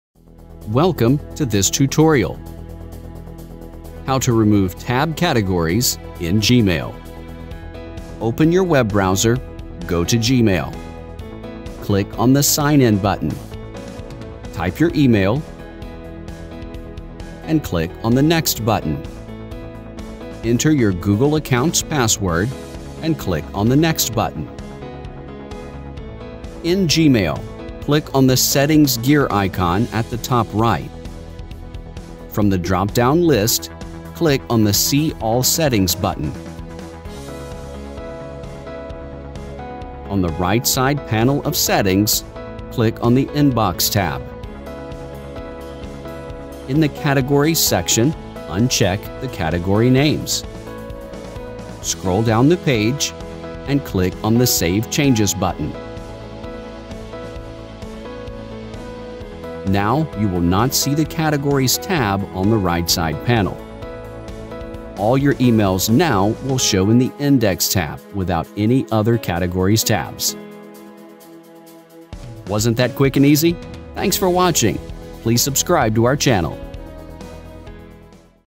How to Remove Tab Categories in Gmail
The category tabs in Gmail help to categorise your emails. There are different category tabs like Primary, Social, Promotions, Updates, and Forums. If you are don’t want to see emails in different category tabs than you disable it.
It is very easy to remove the categories tab in Gmail.
1. Open your web browser, go to Gmail.
2. Click on the Sign in button.
3. Type your email, and click on the Next button.
4. Enter your Google accounts password, and click on the Next button.
5. In Gmail, click on the Settings gear icon at the top-right.
6. From the drop-down list, click on the See all settings button.
7. On the right-side panel of Settings, click on the Inbox tab.
8. In the Categories section, uncheck the category names.
9. Scroll down the page, and click on the Save Changes button.
10. Now, you will not see the Categories tab on the right-side panel.
11. All your emails now will show in the Index tab without any other categories tabs.